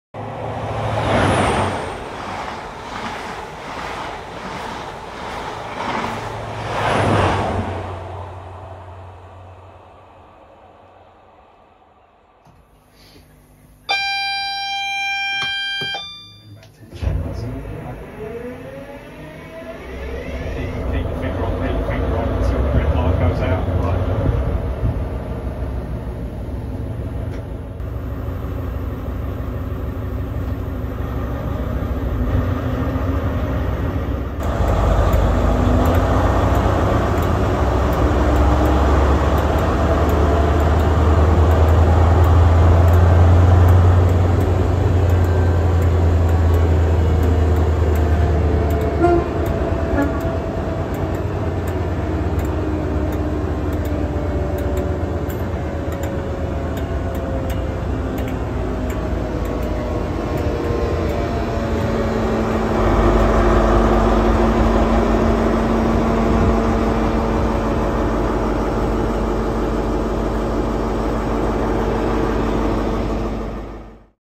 Starting HST Power Car 43071
My first go at firing up Ex. First Great Western power car 43071, courtesy of 125 Heritage. The power car is fitted with the MTU 16V4000 R41R engine, which started up for the first time in preservation without a fault back in February 2022.